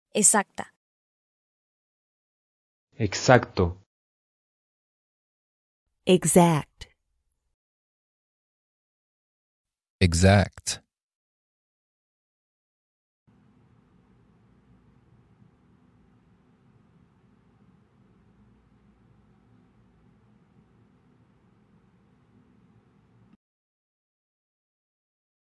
How to say "Exact" in Spanish
Male Voice:
Exacto

Female Voice:
Exacta

Male Voice Translation:
Exact

Female Voice Translation:
Exact